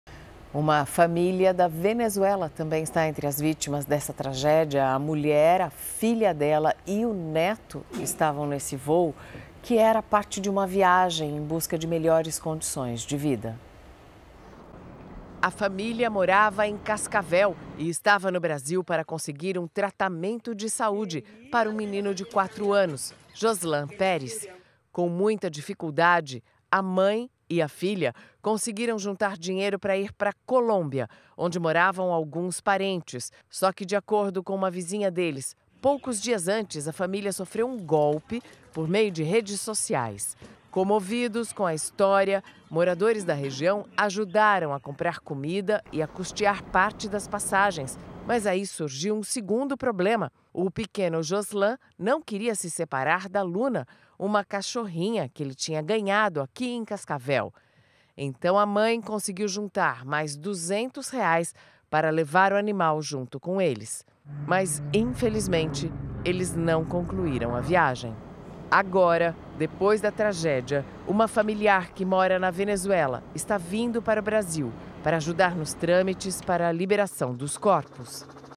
Família da Venezuela é vítima de tragédia do voo 2283
Uma família da Venezuela estava entre as vítimas do acidente aéreo em Vinhedo, interior paulista. A mulher, sua filha e o neto faziam parte de uma viagem com destino à Colômbia em busca de melhores condições de vida. Eles residiam em Cascavel e estavam no Brasil para obter tratamento médico para Joslan Pérez, um menino de 4 anos.